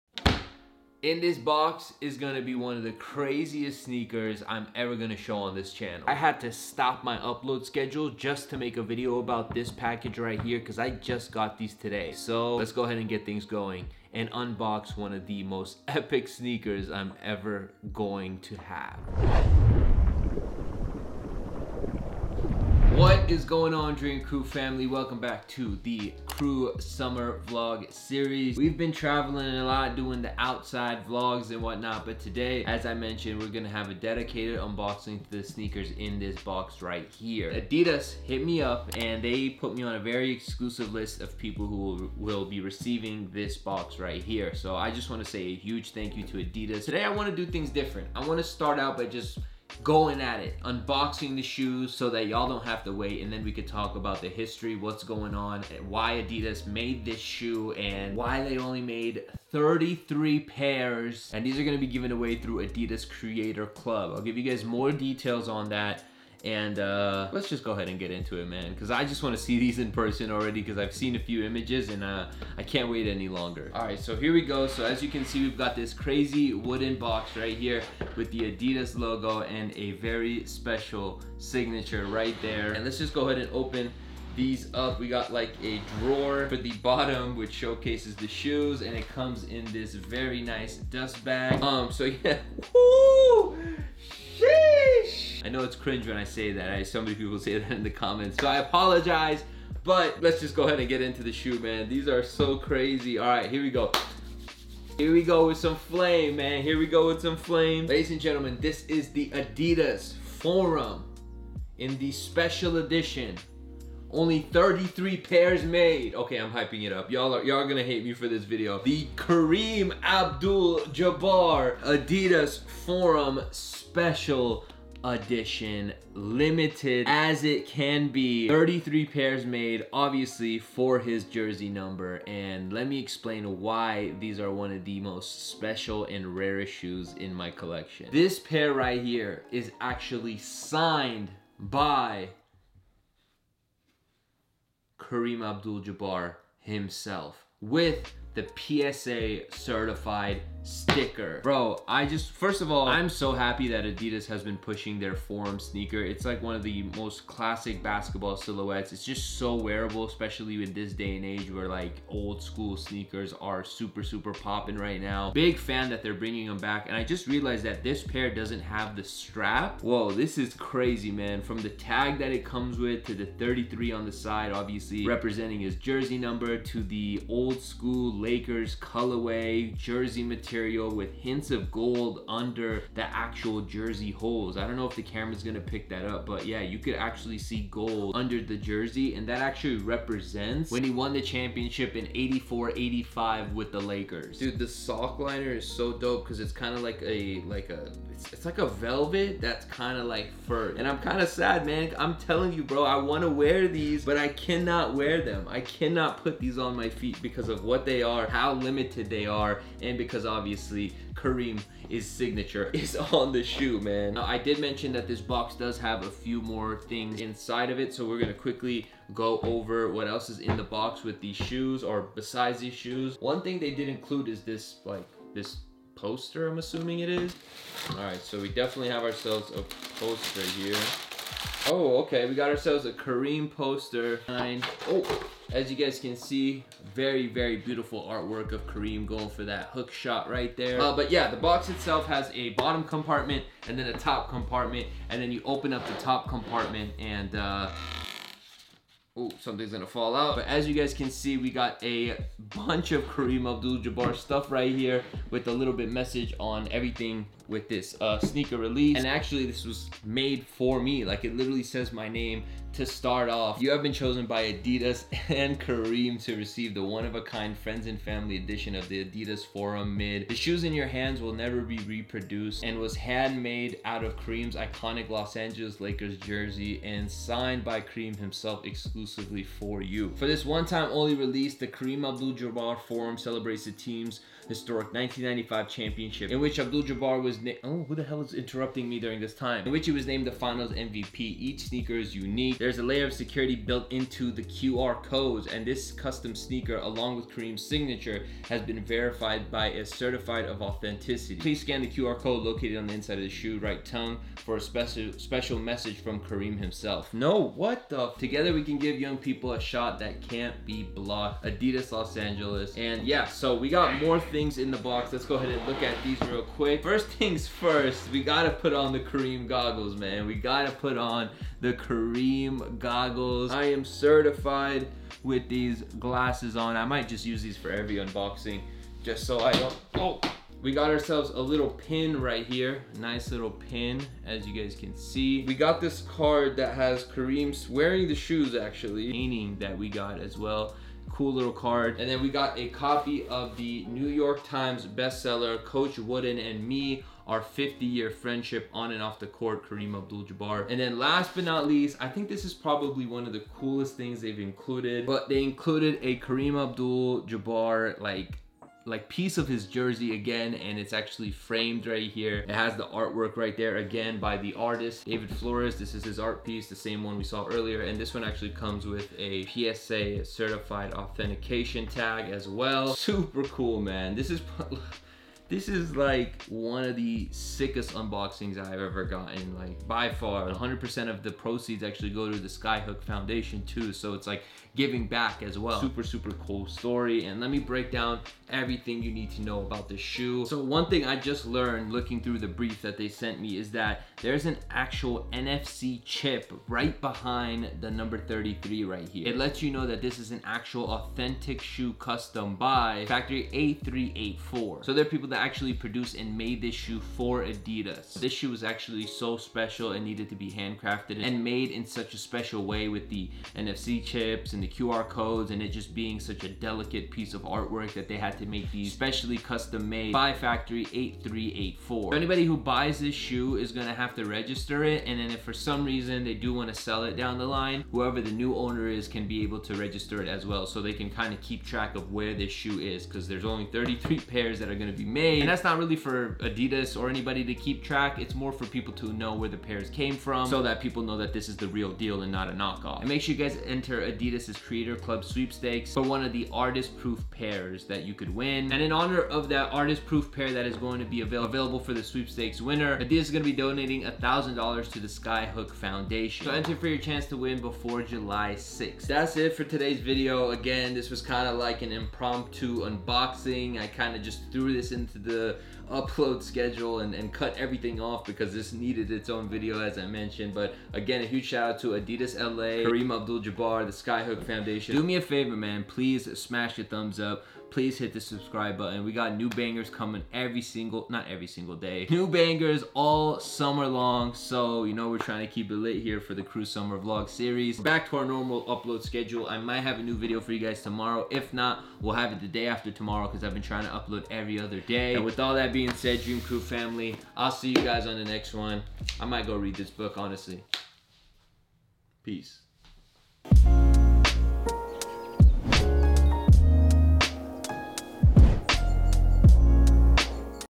THE MOST LIMITED SNEAKER IN MY COLLECTION!! (ONLY 33 PAIRS IN THE WORLD)
Today we unox one of the craziest sneakers in my collection from @adidasLA and @kareemabduljabbar_33 😳